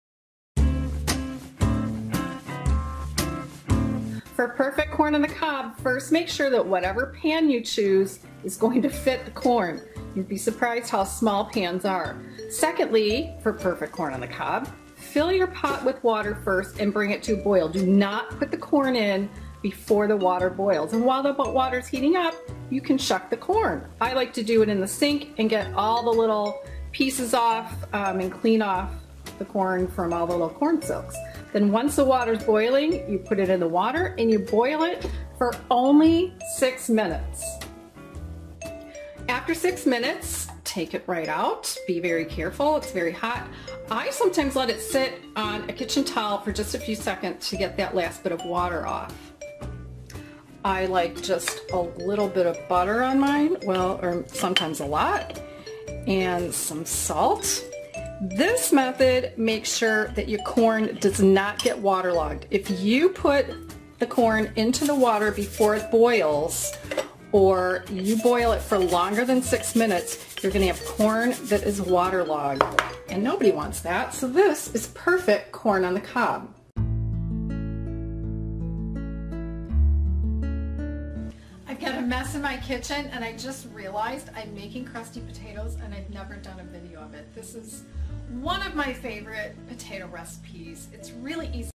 PERFECT CORN ON THE COB! Easy, too. 2 TIPS!
Corn on the cob is easy and turns out perfectly with these 2 tips!

Watch this video to see how to easily cook corn on the cob that is tasty and not water-logged.

Please like/click the “Thumbs up” button and write a comment. I’d like to know what you think, your suggestions for products, what you’ve tried, etc.

Corn on the Cob Holders*
Corn Dishes*